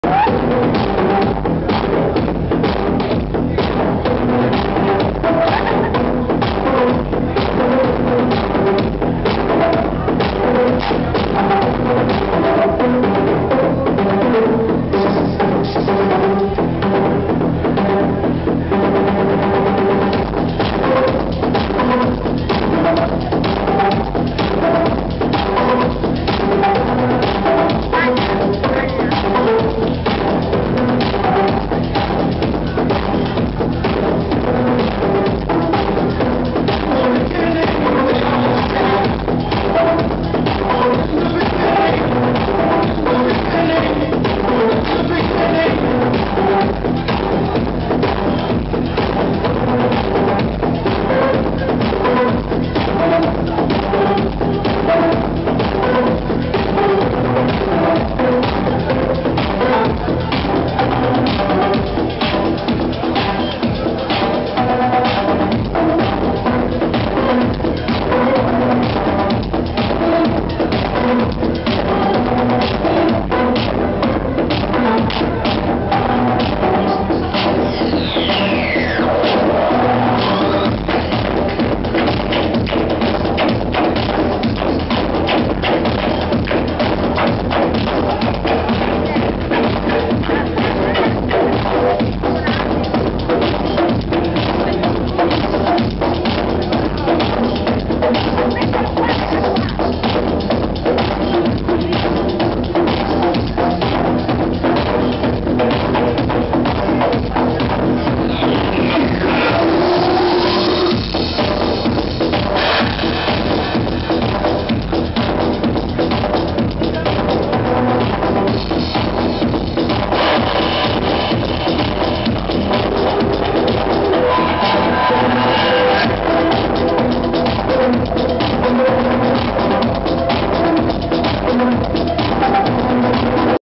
Flock & Mr Paris @ Poker Room
La fantastica prestazione al maffia di reggio emilia di due dei nostri resident dj!